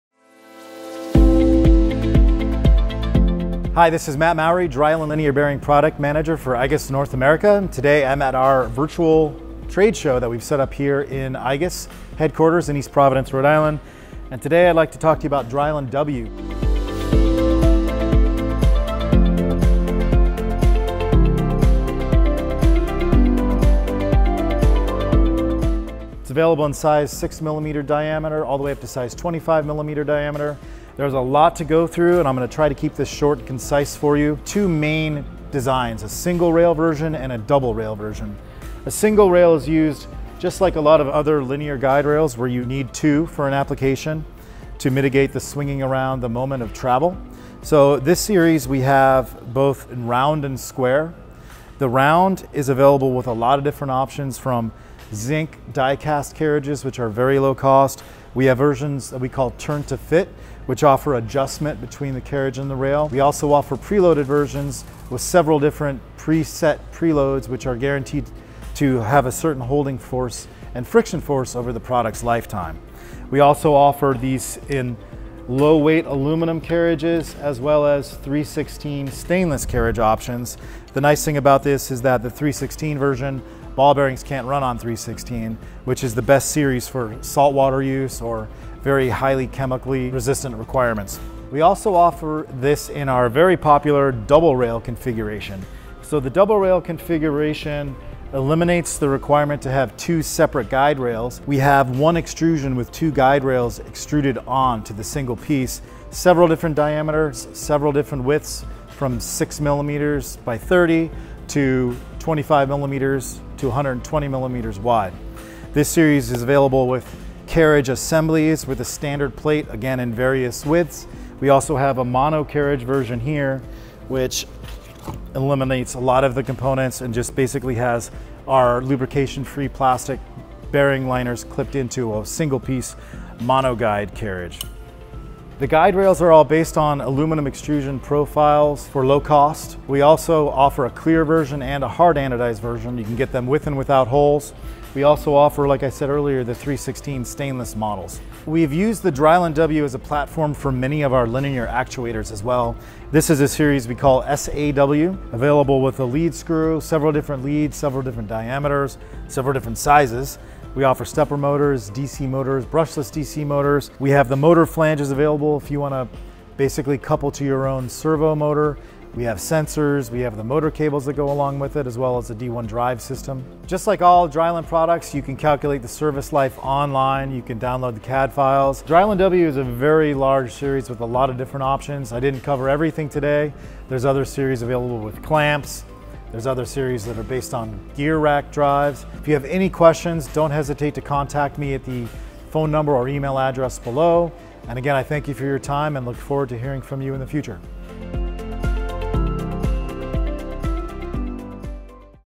Overview - drylin® W linear bearings
drylin® W is a modular linear rail system designed with self-lubricating, maintenance-free sliding plastic liners. We offer single or double rails for design flexibility, various carriage assemblies, and more. Watch this video with drylin® Product Manager Matt Mowry for more information on the product line and its advantages.